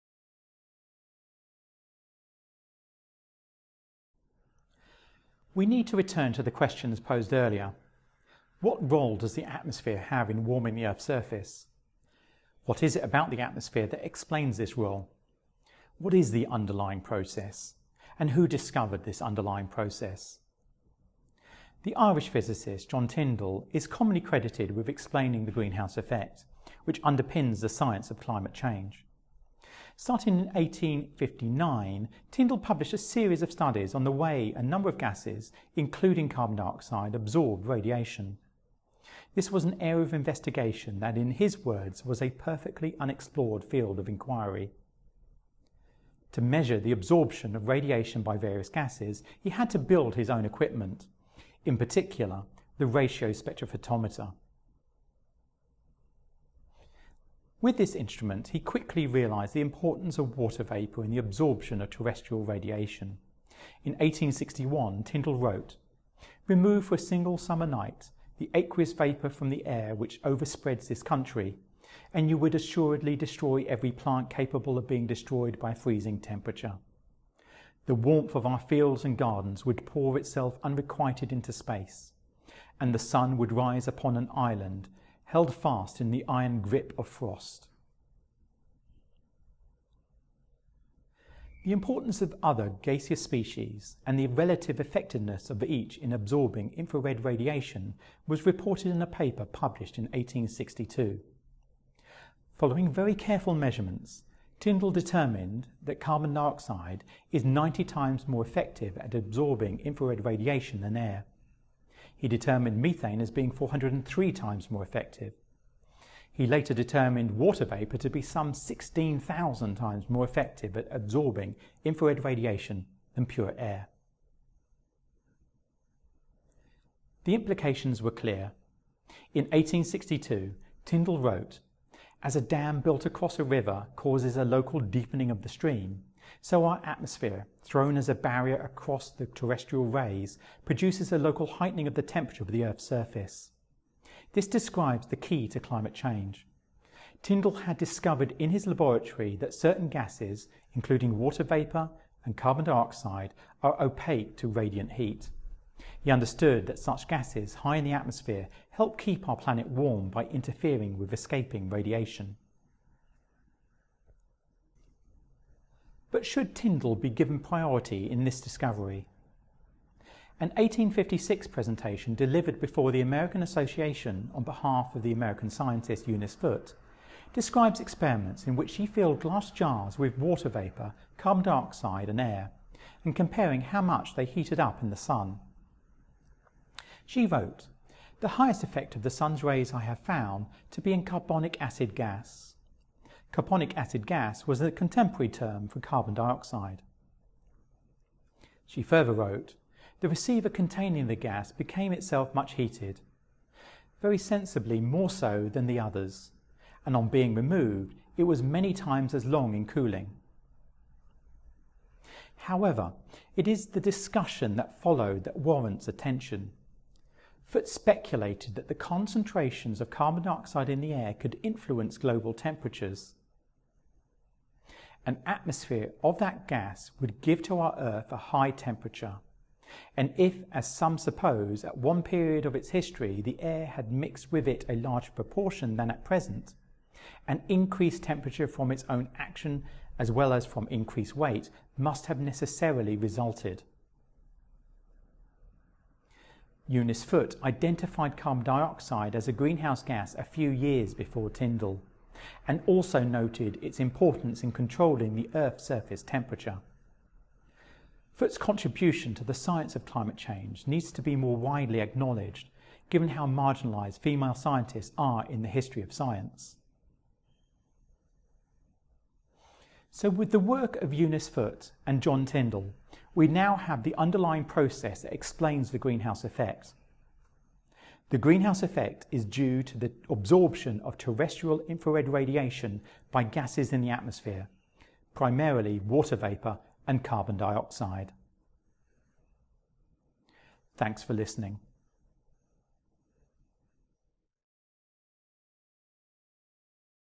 HSI1000 Week 5 Part V: Role of the Atmosphere
This video discusses the development of our understanding of how the atmosphere heats the Earth's surface.

Chapters:
0:00 Introduction
0:21 A perfectly unexplored field of inquiry
0:56 The importance of water vapour
2:06 The impact of the atmosphere
2:50 The invisibility of women in science

Images and videos:
Drawing of Tyndall's ratio spectrophotometer published in Phil. Trans. R. Soc.151: 1–36 — public domain